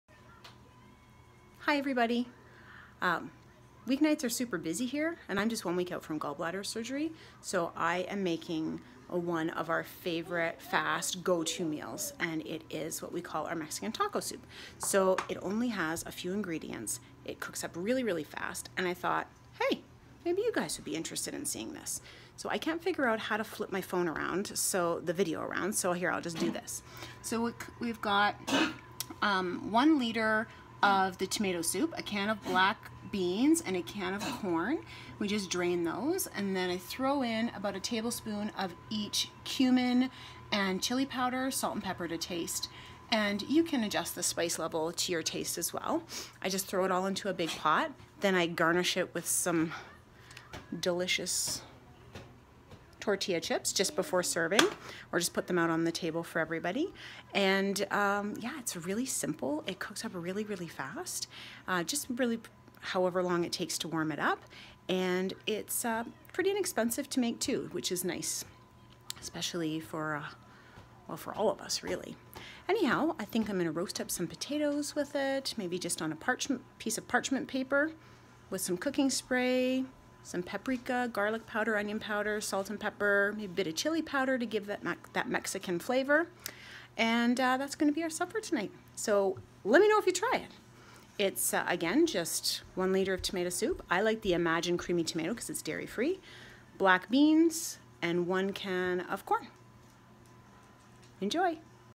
Easy plant based lunch
One of our favourite weeknight dinners. Garnish with a few tortilla chips. This meal is easy, fast, fairly inexpensive, and delish.